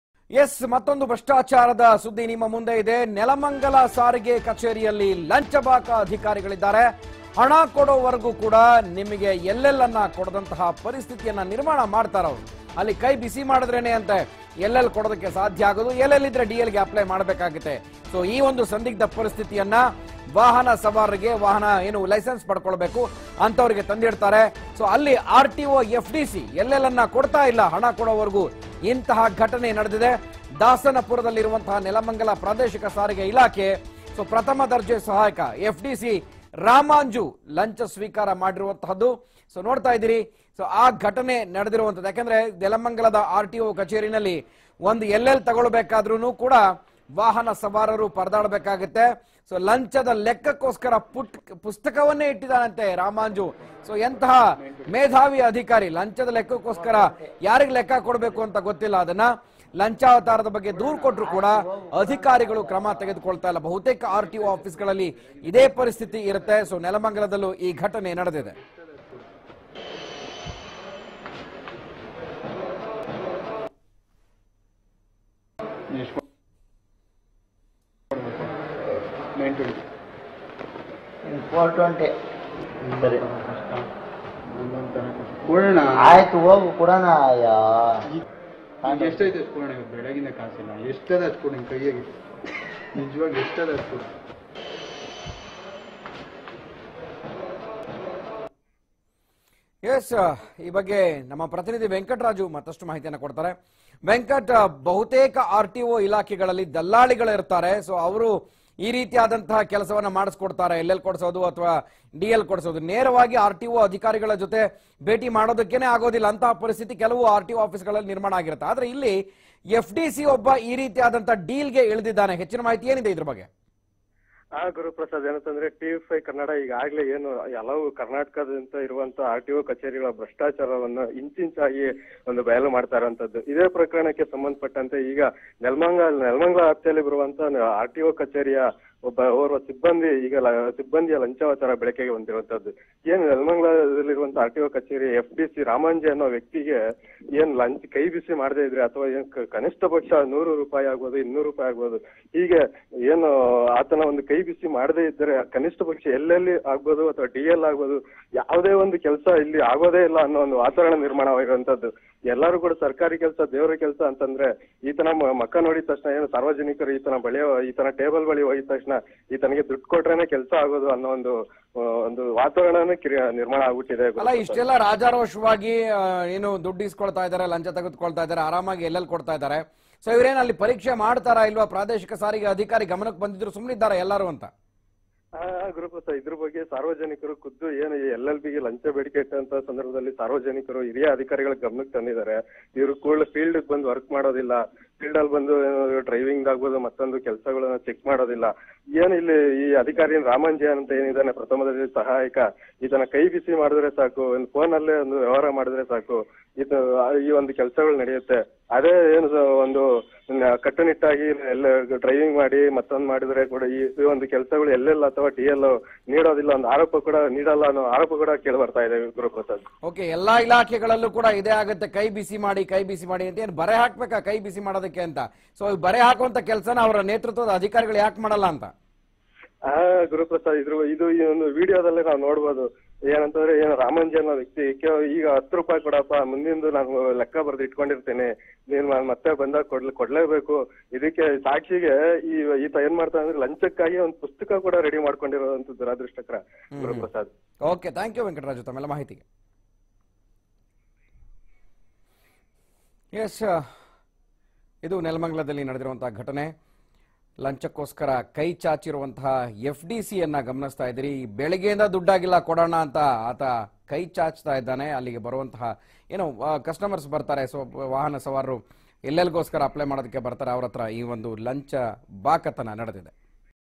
LLR for Just Rs.420 /- | Corruption at Nelamangala RTO Office | TV5 Kannada
Ruthless Corruption at Nelamangla RTO Office Ramanji FDC at Nelamangla RTO.